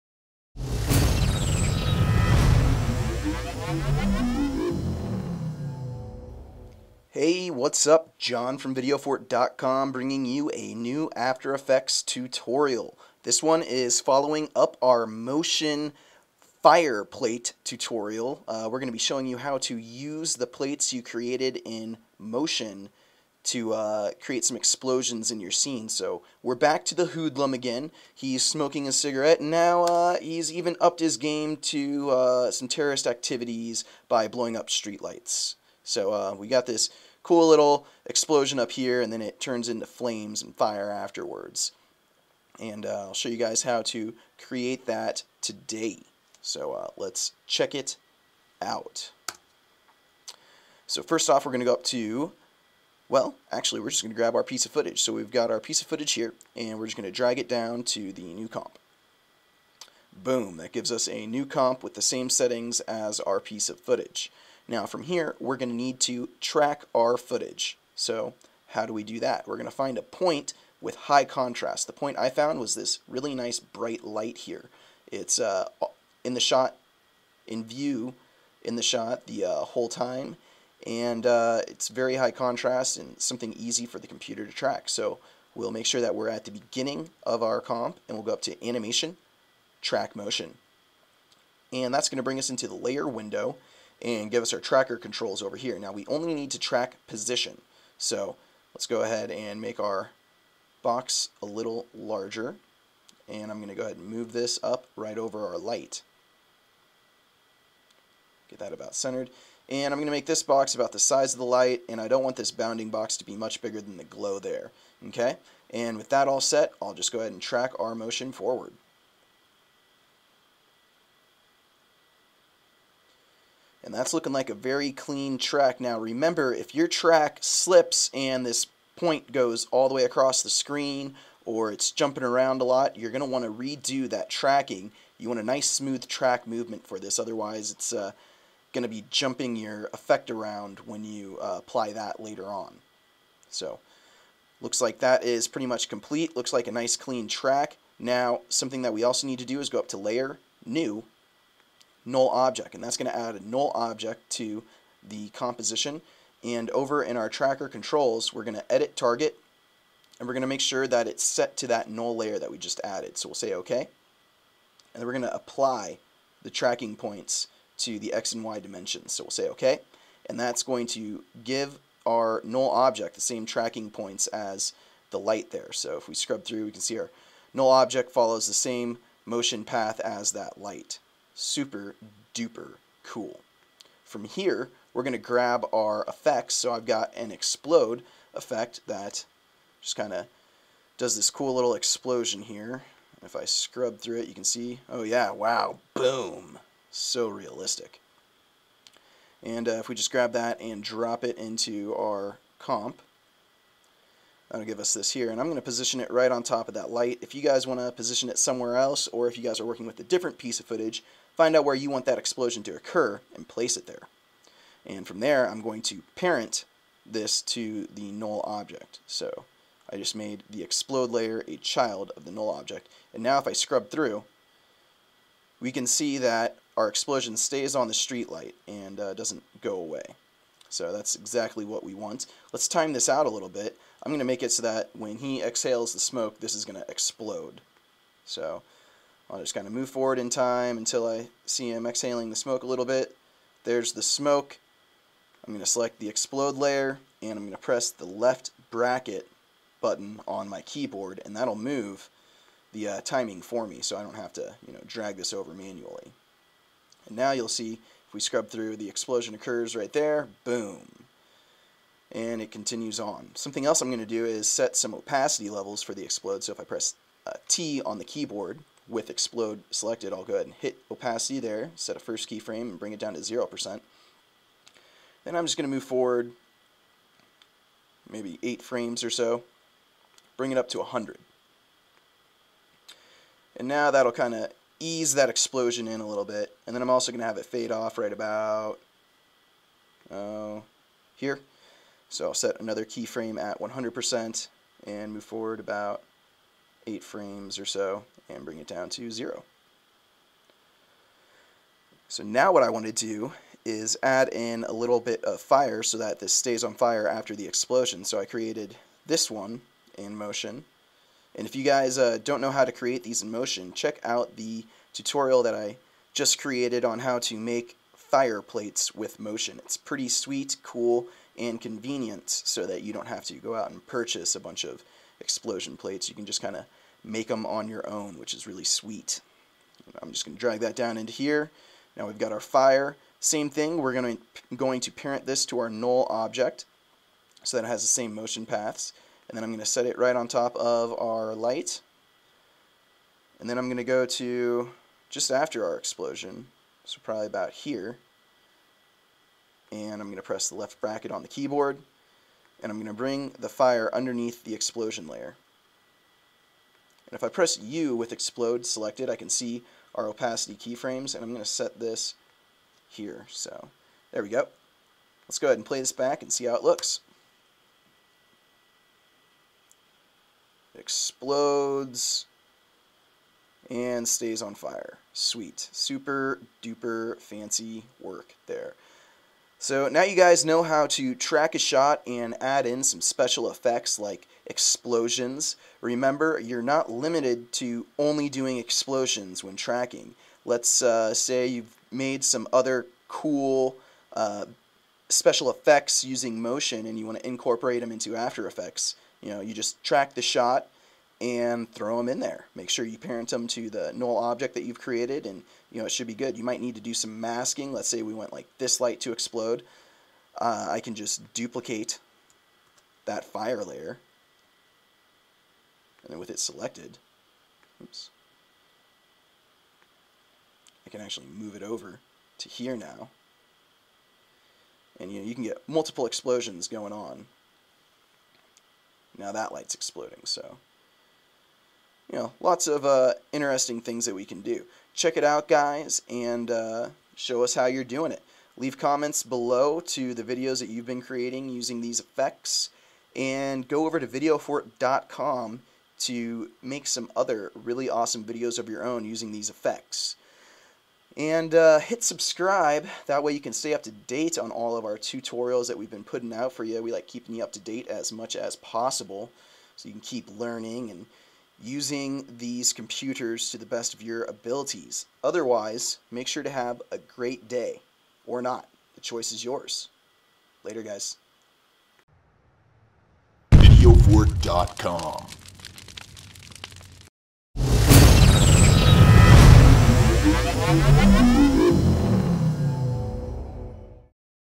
After Effects: How to Apply Explosion Plates
In this After Effects Tutorial VideoFort Jon show you how to apply Explosion Plates, created in Apple's Motion, to a piece of footage. Start out by dragging your piece of footage to the new comp button. Then go to Animate_Track Motion and select a high contrast area to track the motion. After you have tracked the motion add a Null layer and apply the tracking data to the Null object. Now import the explosion plate to your comp and parent the explosion to the null object. Place the explosion whereever you want in the comp and time it as needed. RAM Preview to playback your comp and you should have a nicely tracked explosion! What cool effects videos do you create? Post them in the comments below!